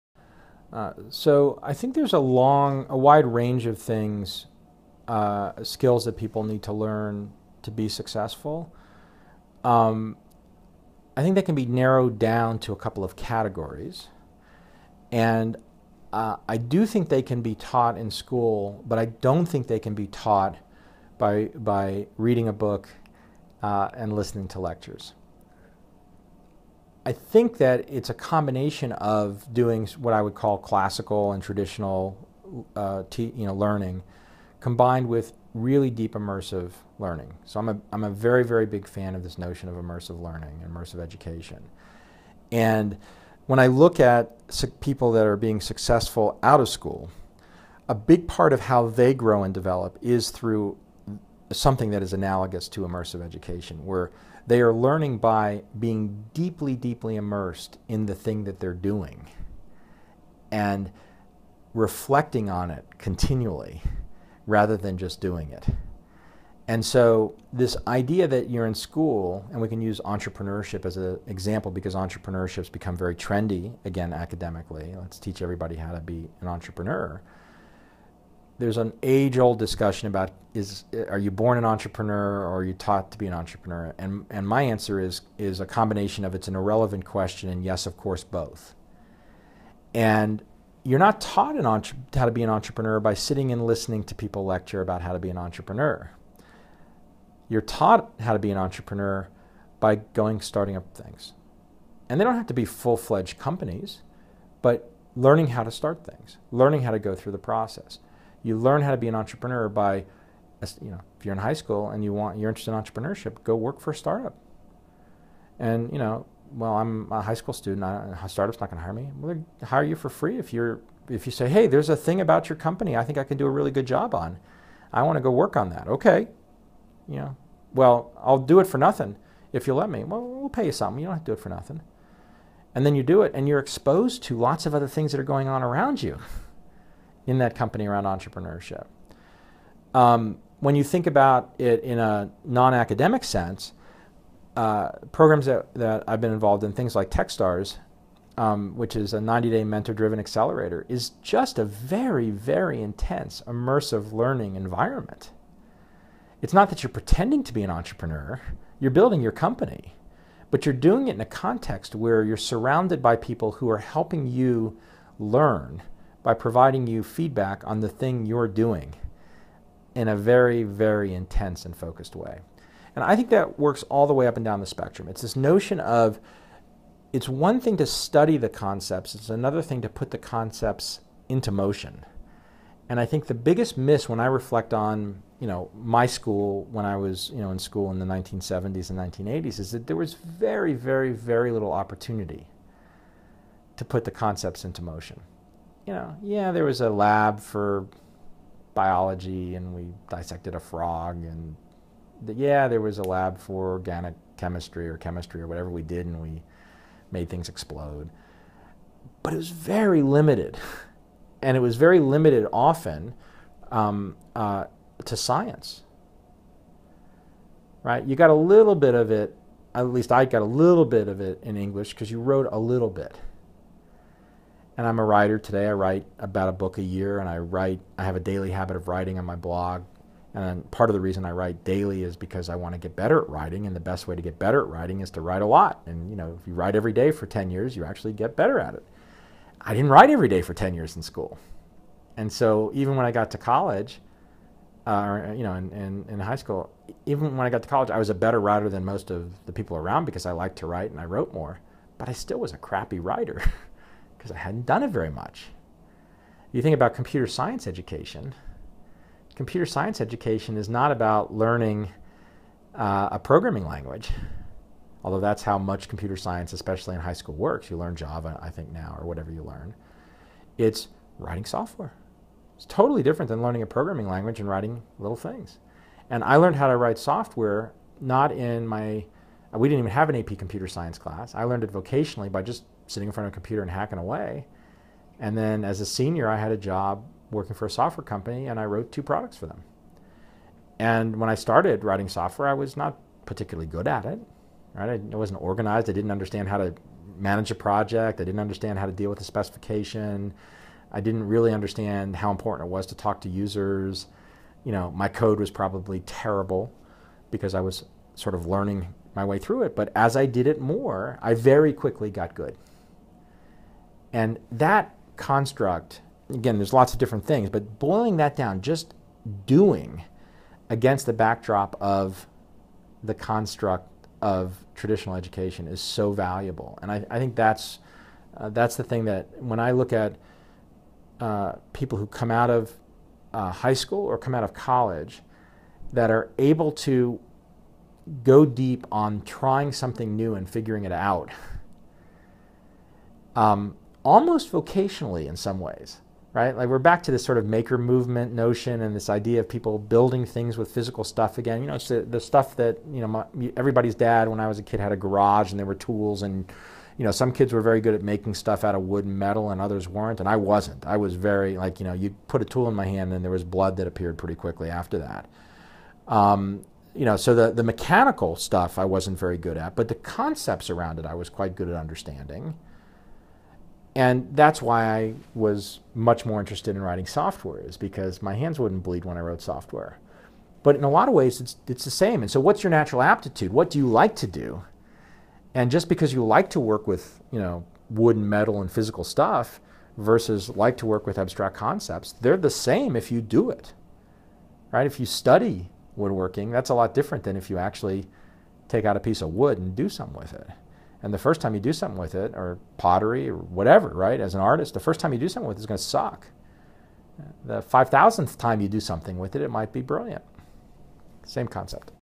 Immersive Learning
Brad Feld explains why immersive learning is critical for kids today and why education must change.